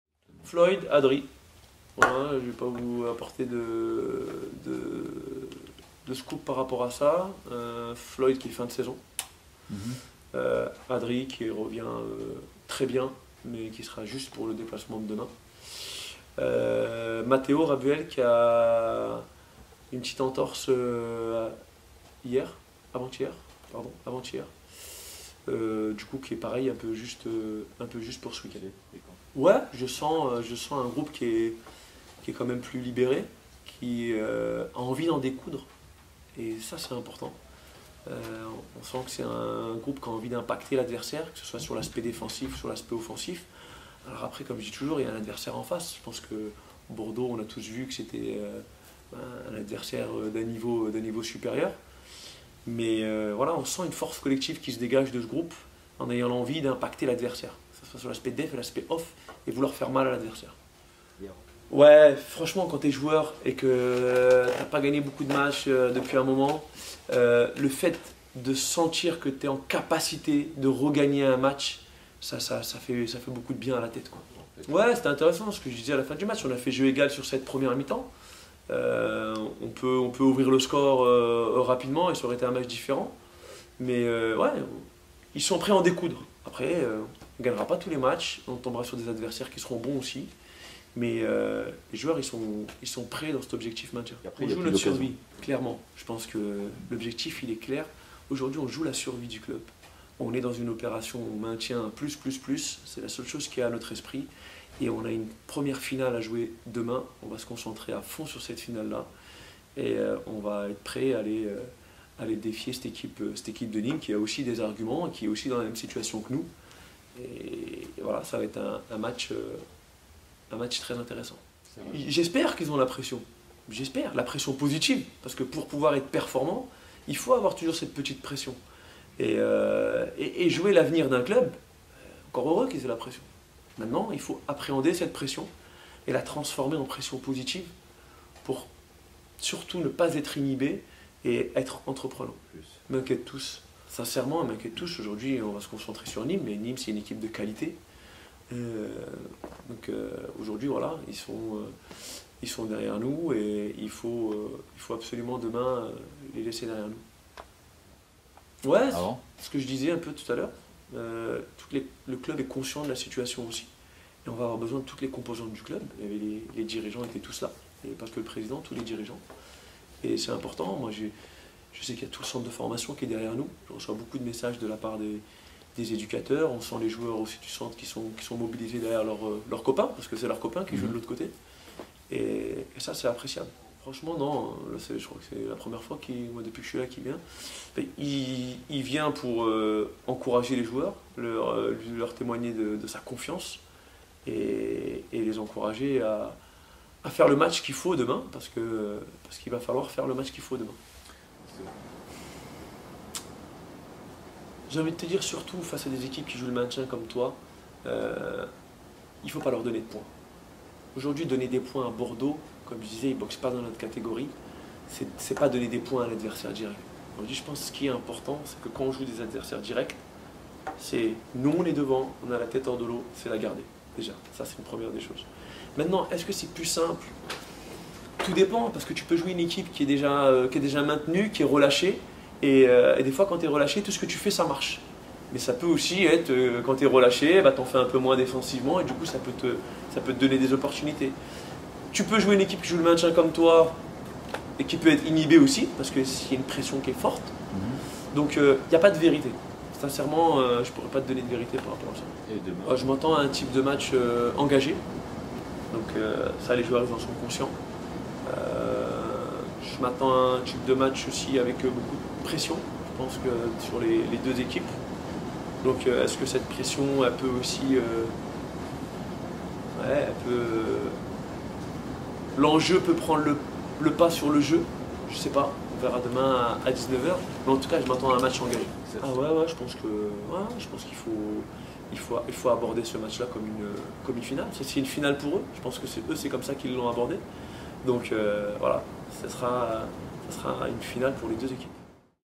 🎙️ Conférence de presse d'avant match | Nîmes - VAFC
🎥 La Conférence de presse avant Nîmes - VAFC

🎙Ahmed Kantari : “Il faut aborder ce match comme une finale, ça sera une finale pour les deux équipes.” 🔴⚪️